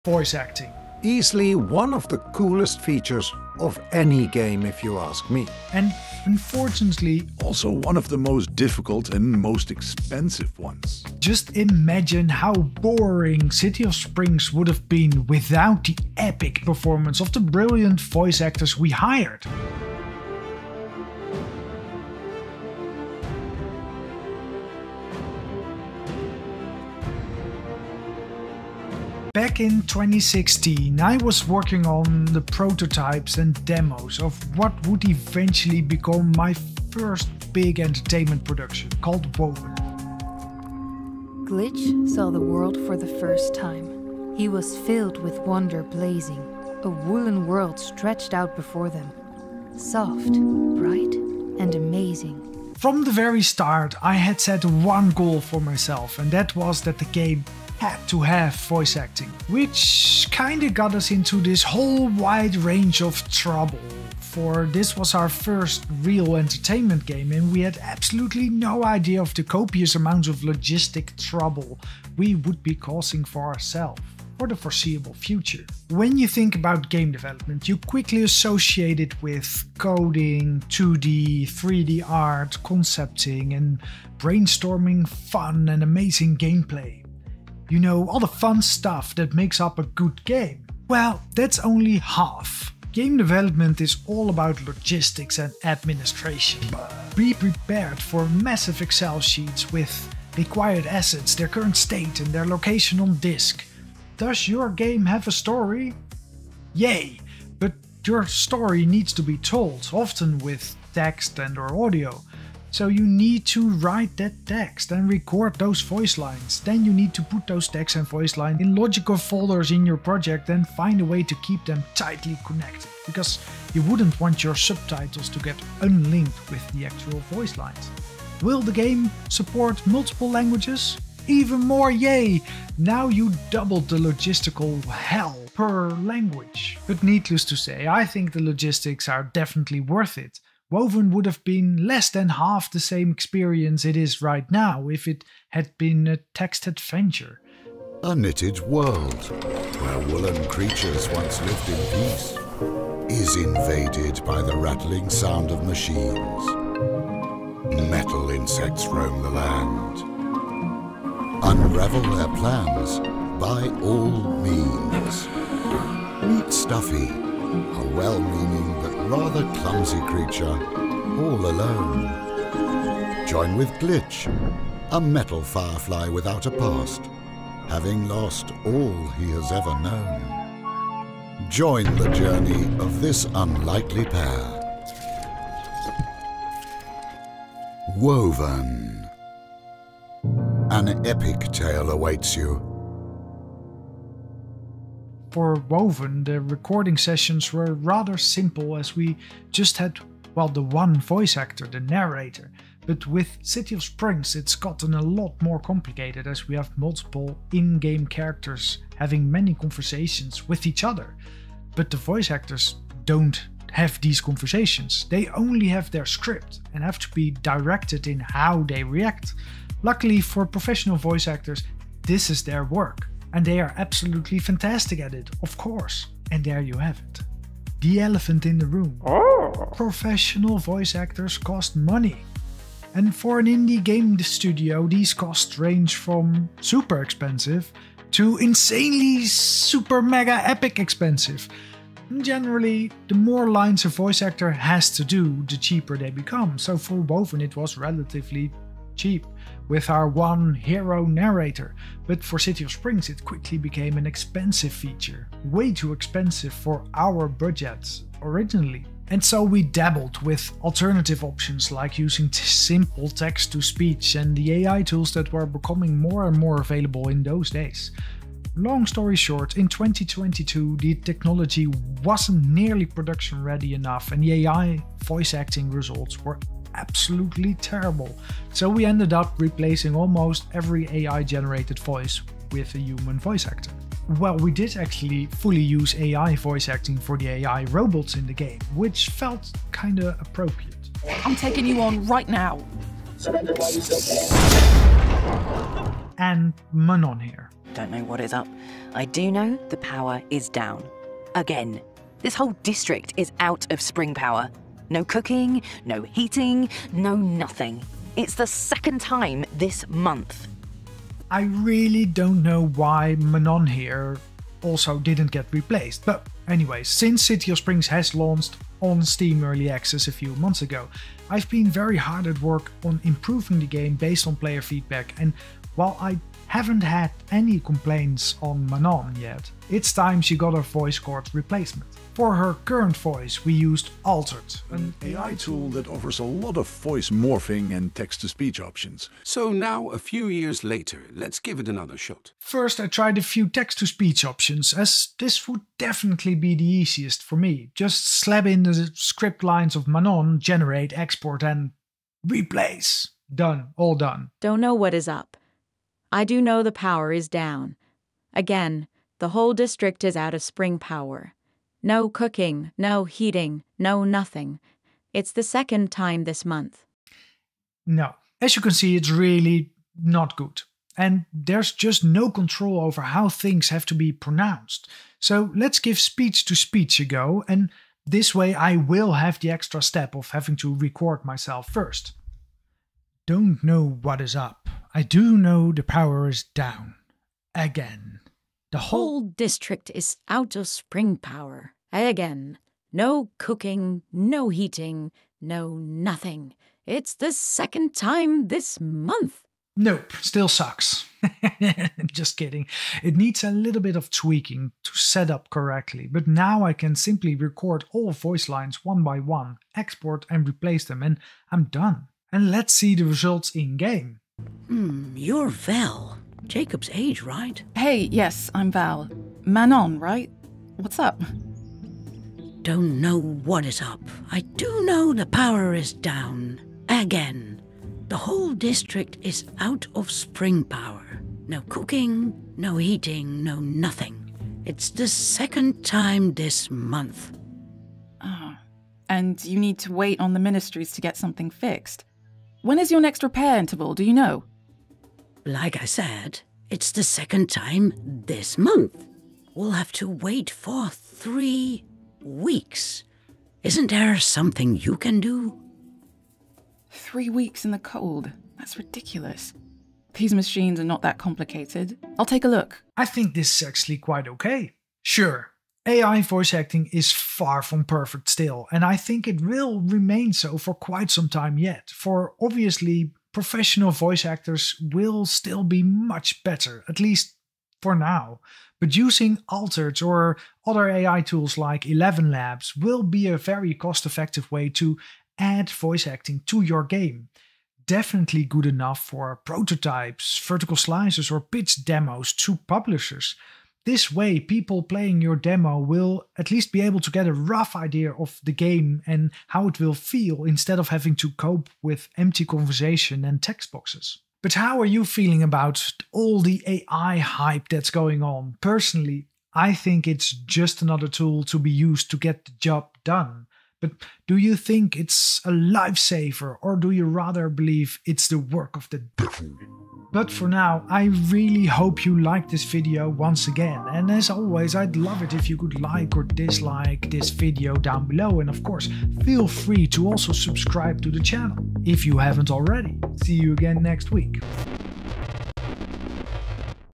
How to use AI for Voice Acting - Game Dev
Did you know there are AI tools that you can use to your game's voice acting with? In this video I test how production ready they are and use them for my own game City of Springs!

00:00 Voice Acting
00:29 Difficult
02:17 Recording
03:46 Cost
04:19 AI
04:57 2025
07:45 TL;DR
08:24 HYPE